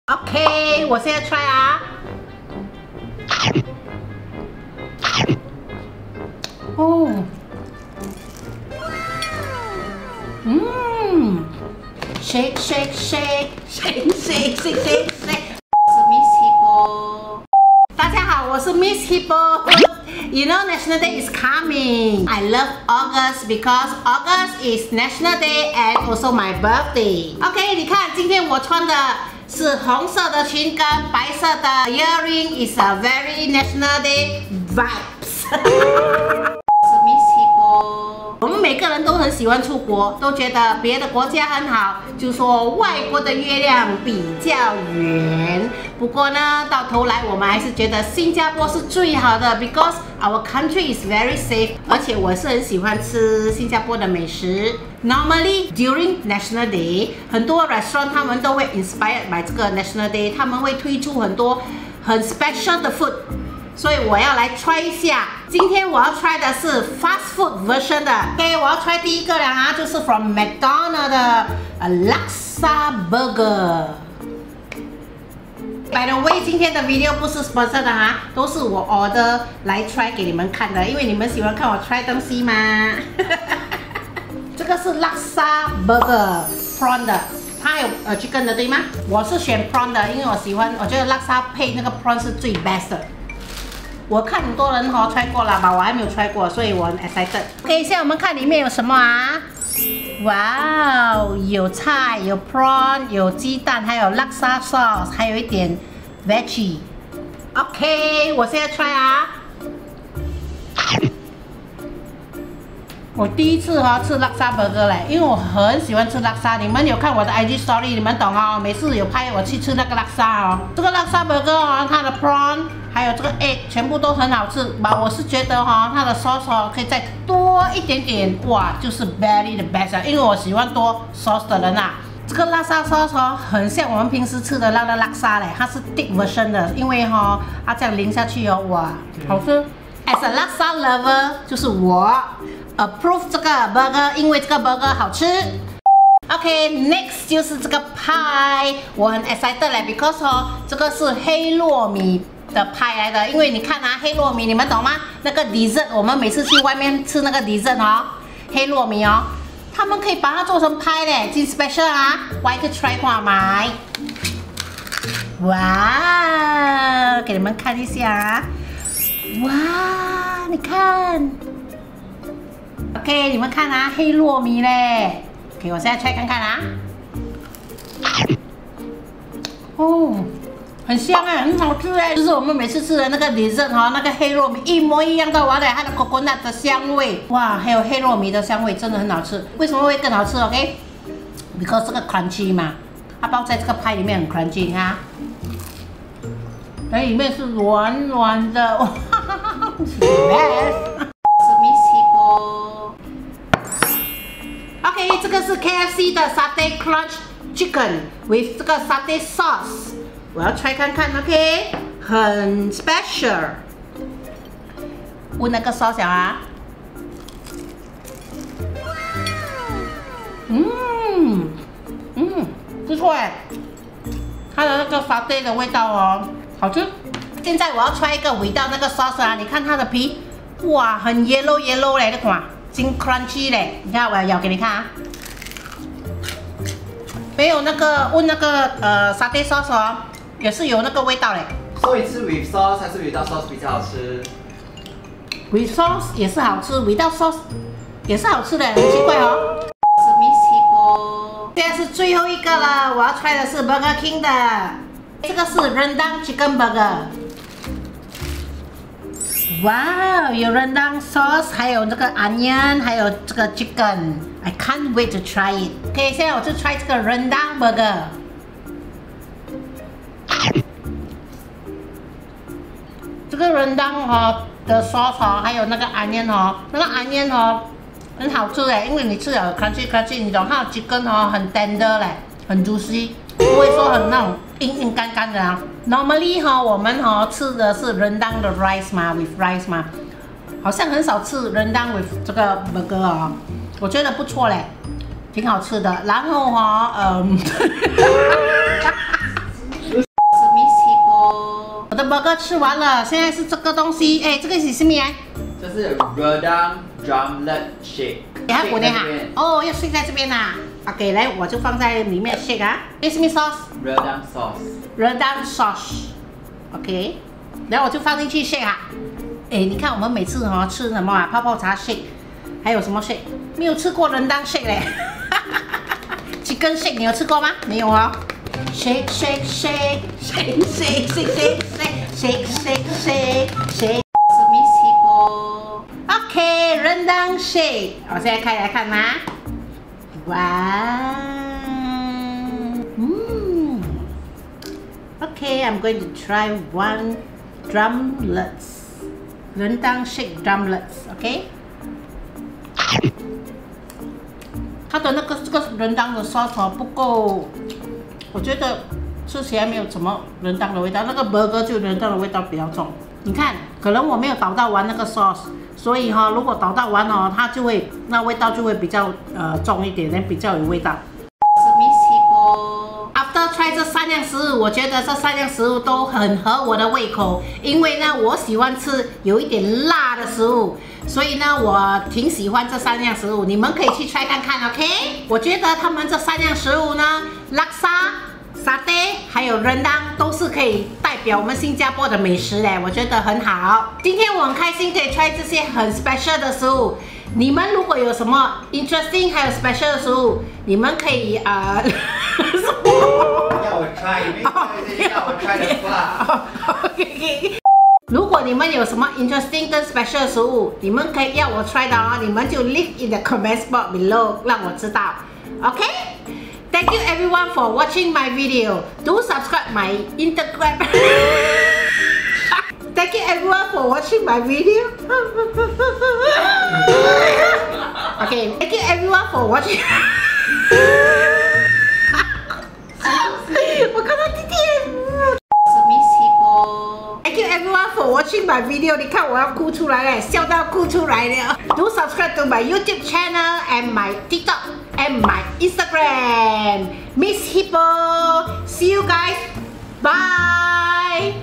EATING NATIONAL DAY SPECIAL FAST FOOD MENU *SO NICE* 吃新加坡国庆日特别快餐菜单 *很好吃*
Happy National Day, Singapore! Miss Hippo 来吃新加坡国庆日特别快餐菜单咯！快来看！Miss Hippo celebrates National Day by trying all the fast food special national day menu!!! Watch to find out more 🙂